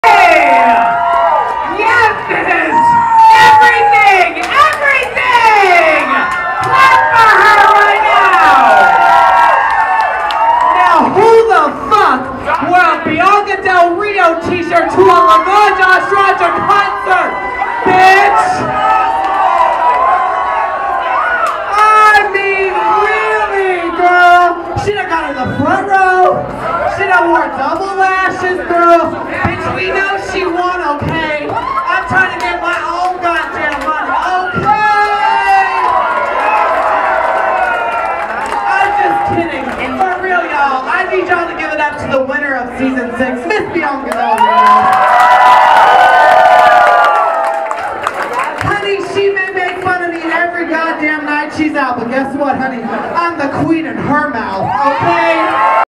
Laganja Estranja Vs Bianca Del Rio T-Shirt On Boston Fan @ Show (2014)
Laganja Estranja From RuPaul's Drag Race Season 6 Spots A Boston Fan In Audience Wearing Season 6 Winner Bianca Del Rio T-Shirt @ Her Boston MACHINE Nightclub Show Hosted By Lili Whiteass WA. Recorded September 12, 2014 By Diamond Dunhill On Location @ MACHINE Nightclub, Boston MA.
Special Shout-Out To Bianca Del Rio, Winner Of RuPaul's Drag Race Season 6 As Seen On LOGO TV.